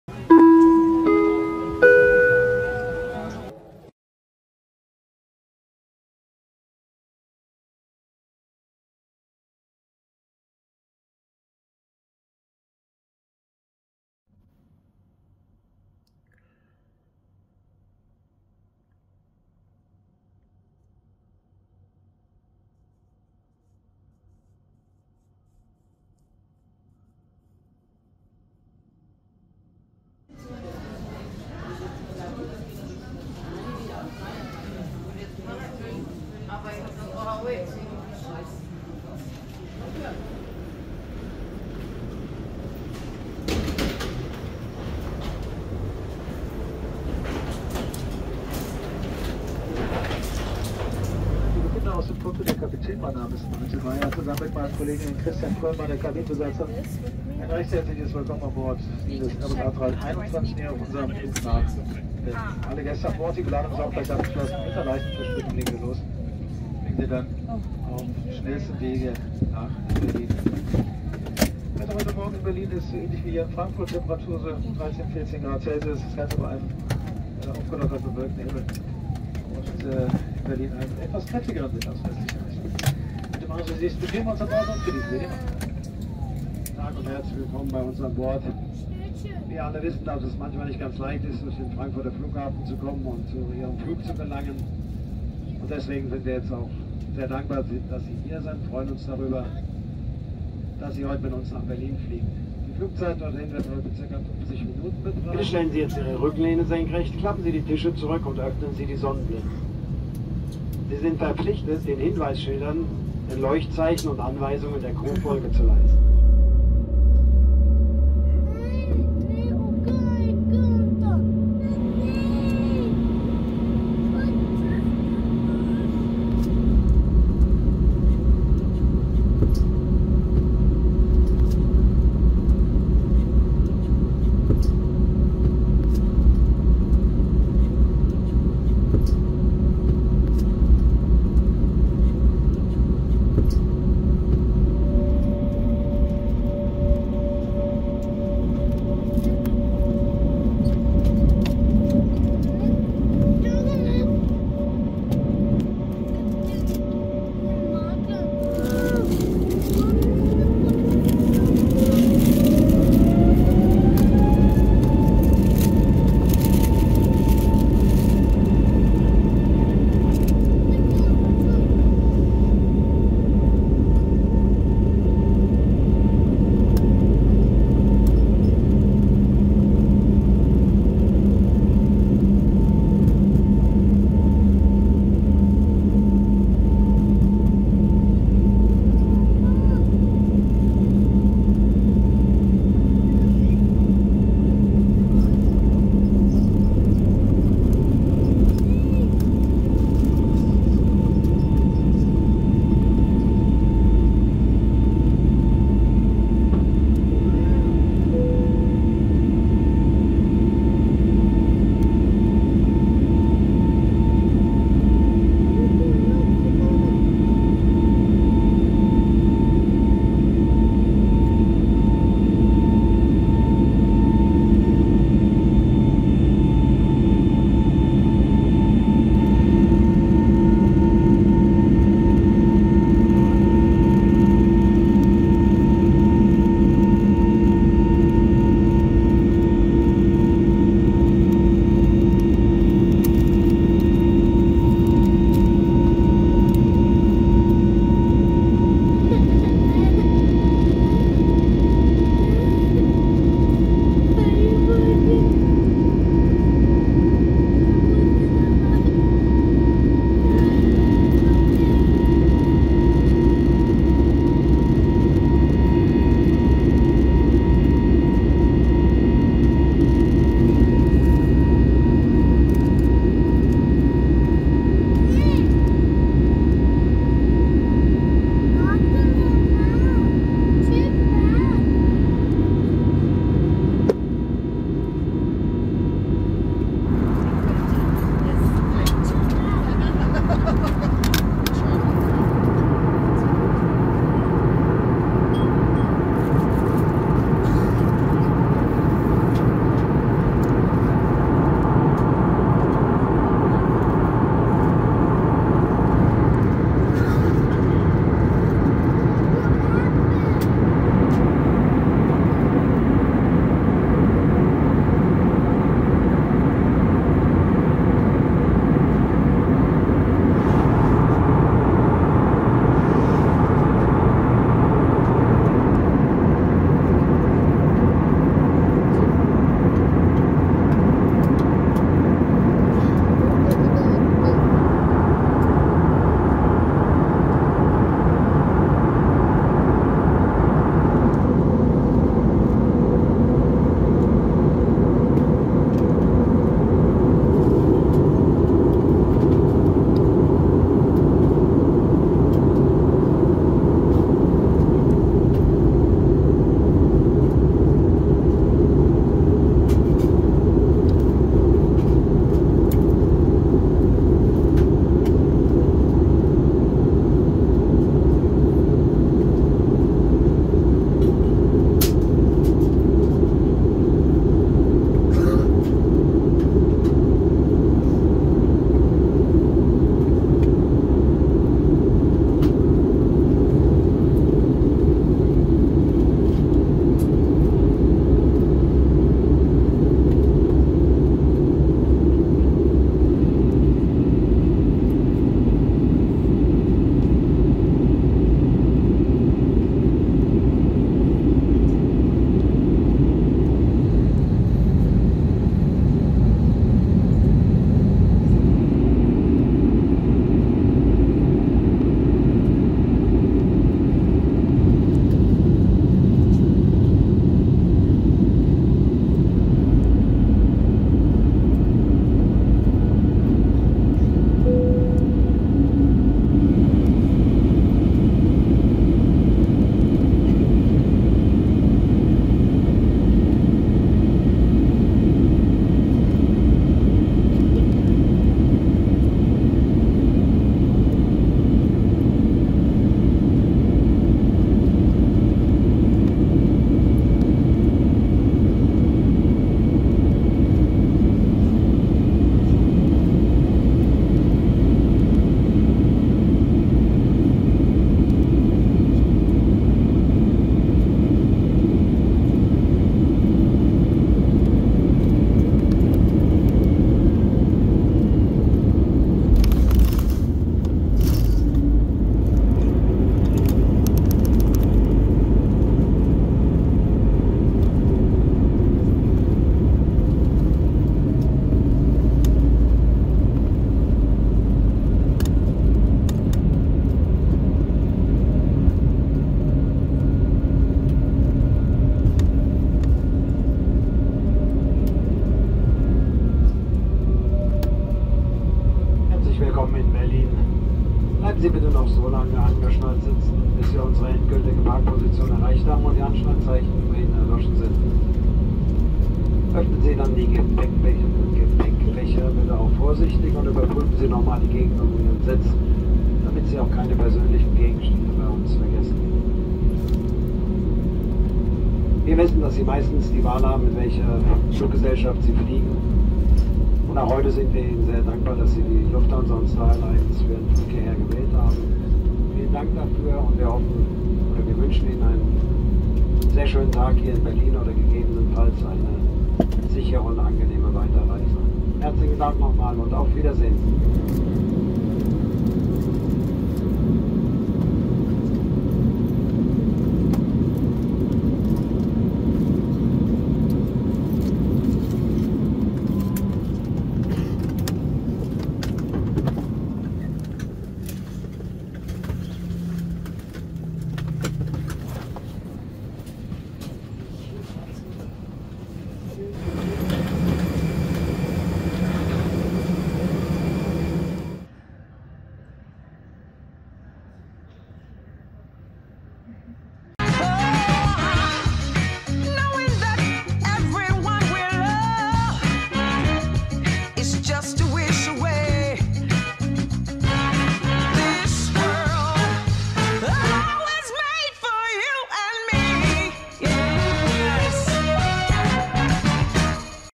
TRIPREPORT 4K | Lufthansa Business Class | A321neo D-AIEJ | Frankfurt/Main - Berlin | 28.09.2024
Willkommen zu diesem neuen TRIPREPORT (NR. 91)

Heute fliegen wir zum 1. mal A321neo zurück nach Berlin.
Flugdetails:
Airline: @Lufthansa
Flugnummer: LH 182
Typ: Airbus A321neo
Reg.: D-AIEJ
Sitzplatz: 7A (Business Class)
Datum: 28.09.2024

Infos:
Kamera`s:  Dashcam: AZDOME GS63H
  Smartphone: Samsung Galaxy S21 Ultra
  Cam: Sony FDR-AX 43
Schnitt:  Sony Vegas 17 Platinum
Kapitel:
0:00 Intro
0:15 Flughafen Frankfurt/Main
0:40 Bus Boarding Gate A13
1:45 Pushback / Taxi to Runway 18
2:40 Takeoff Runway 18
4:45 Cruising FL310
5:20 Decending
7:10 Final Runway 25R
8:15 Taxi to Gate B20
9:15 De-Boarding
9:40 Outro